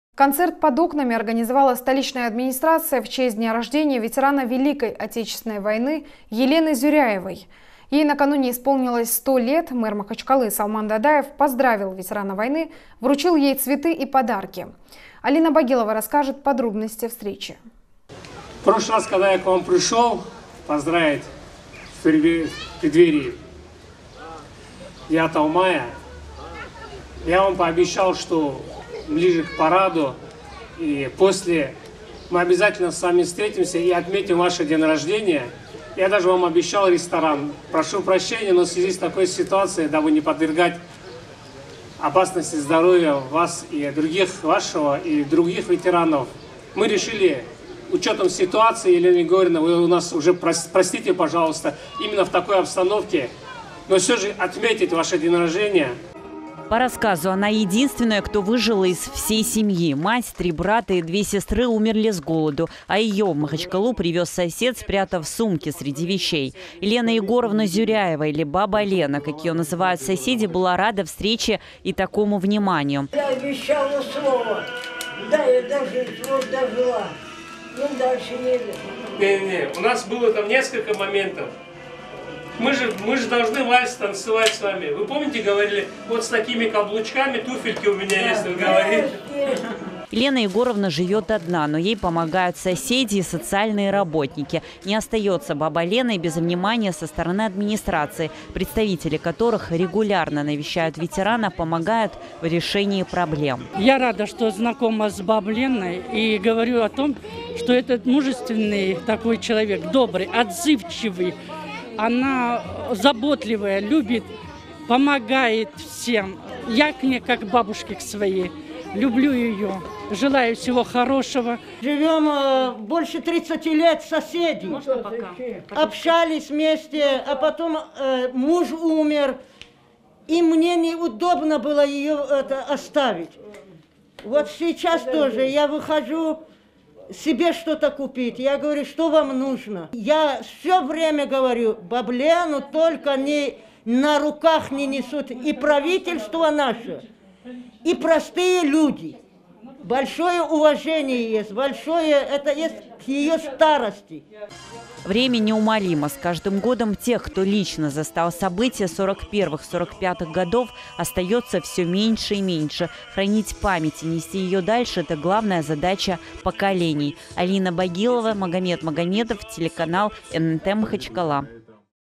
Праздник у дома. Мэр Махачкалы приехал поздравить ветерана
Администрация Махачкалы организовала уличный концерт в честь дня рождения ветерана Великой Отечественной войны Елены Зюряевой перешагнувшей столетний рубеж.

Мэр Махачкалы Салман Дадаев поздравил ветерана войны, вручил ей цветы и подарки, подробнее расскажет наш корреспондент Алина Багилова.